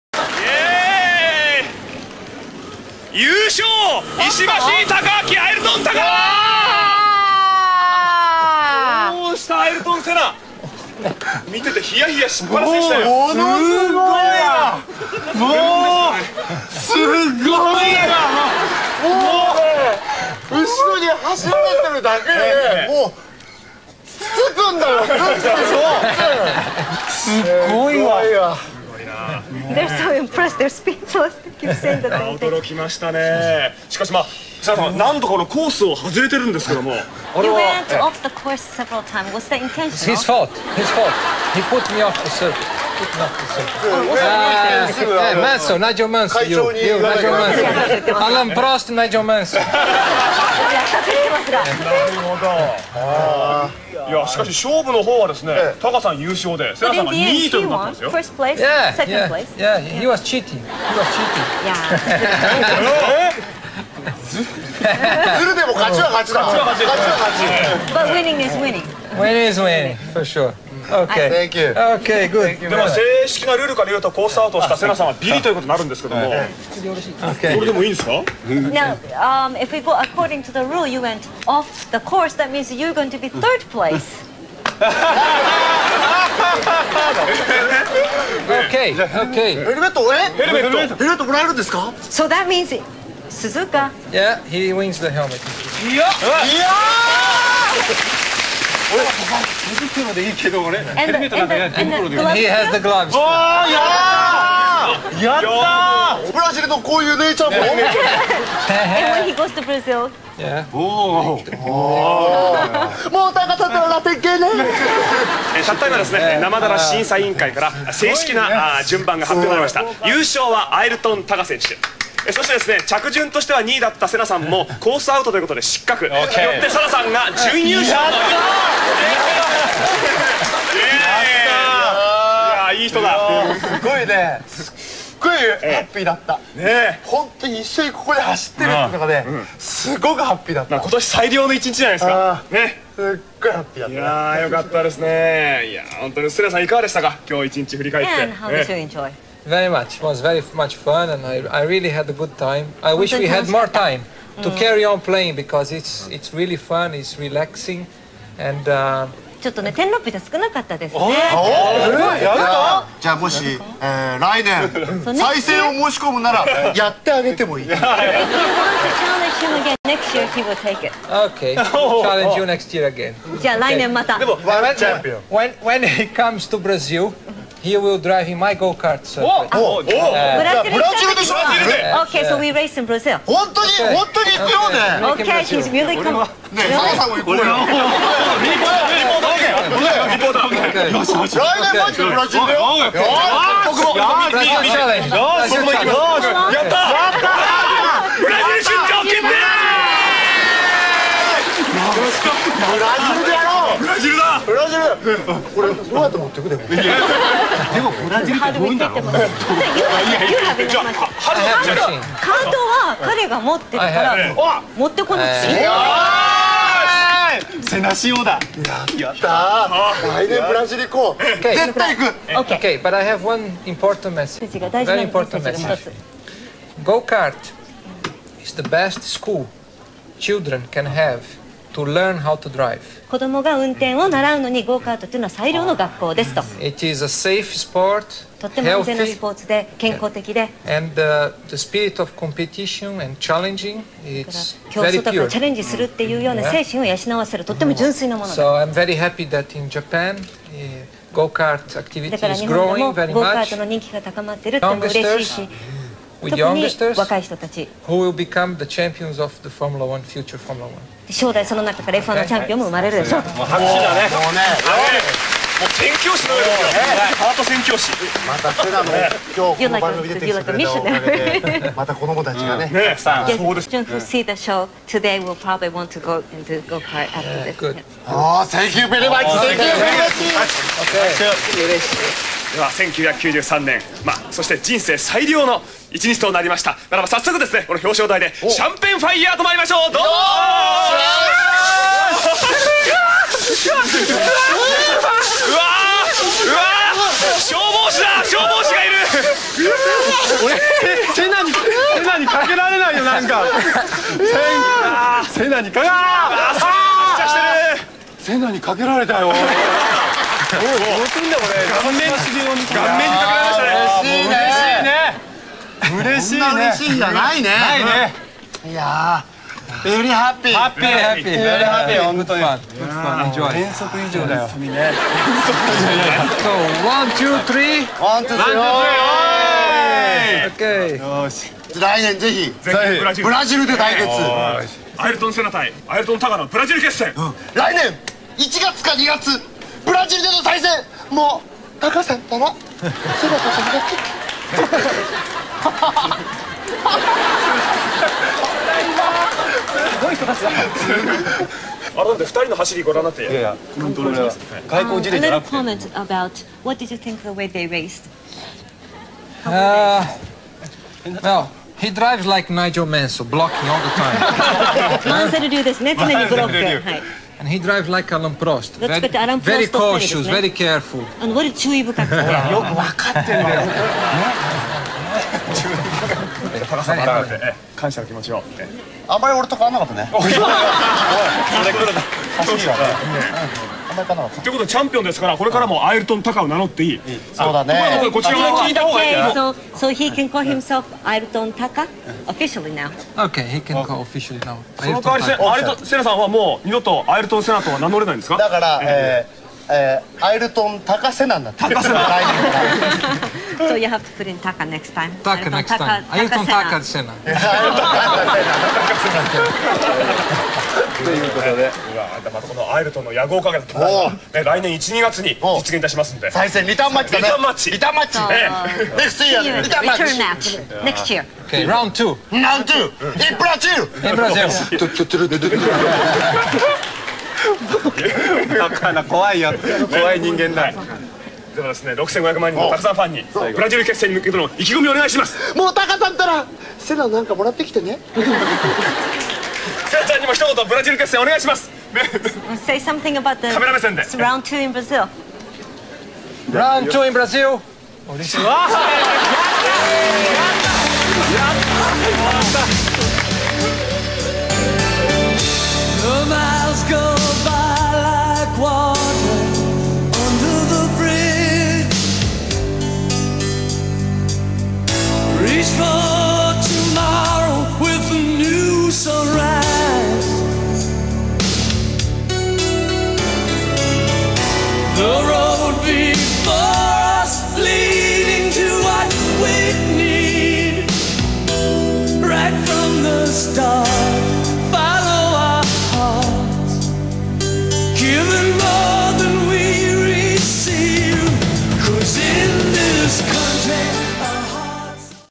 Ayrton Senna on Japanese TV '93 (4/4) ｱｲﾙﾄﾝ･ｾﾅ
I wanted to watch their return match in Brazil 1994.
If they had that in Janualy '94,Did God save Senna??
God should change the past.
God made all the world cry.